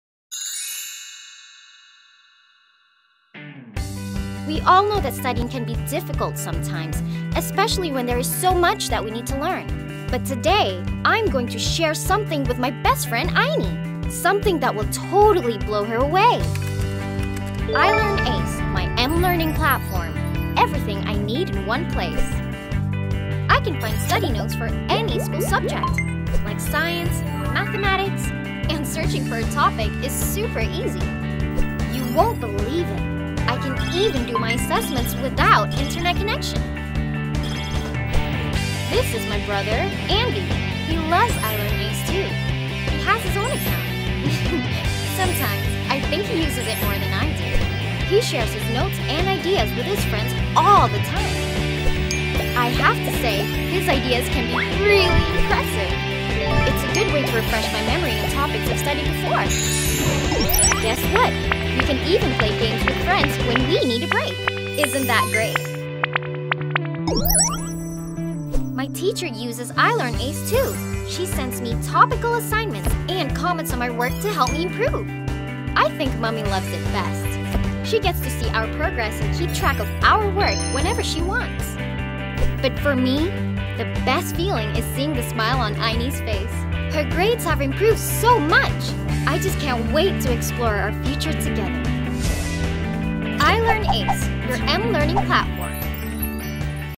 i-LEARN Ace - Top online and offline learning platform for UPSR, PT3 & SPM students in Malaysia.
i-LEARN Ace - Top online and offline learning platform for UPSR, PT3 and SPM students in Malaysia.   马来西亚最尖端的 UPSR, PT3 和 SPM 在线和离线M-学习平台！

i-LEARN Ace is specially designed by SASBADI, a well-known education publisher with 35 years experience, for standard 4 to Form 5 students in Malaysia.
i-LEARN Ace 是35年著名教育出版社 SASBADI 特地为我们 4年级 至 Form5 学生量身定做的一个网络学习平台!

In one account it contains the following:
1个平台里包含以下

✅ Notes  学习笔记 📚
✅ Mindmap 思维导图
✅ Objective practices  习题
✅ Essay and subjective practices  作文习题
✅ KBAT/HOTS and iThink 习题
✅ Pentaksiran Sekolah  校本评价 (PS) (UPSR & PT3 package 配套)
✅ Oral Test 评估 🗣👂🏻
✅ Test Paper, Kertas Model, Kertas Percubaan 评审试卷、UPSR、PT3 和 SPM 预考试卷和模拟试卷 📝
✅ Video lessons 教学视频 (*SPM Form 5 package 配套)
✅ Dictionary 词典 📘
✅ Progress analysis report 分析表报告 📊
✅ Rewards 奖励 💰🎁
✅ Offline apps 移动应用程序 App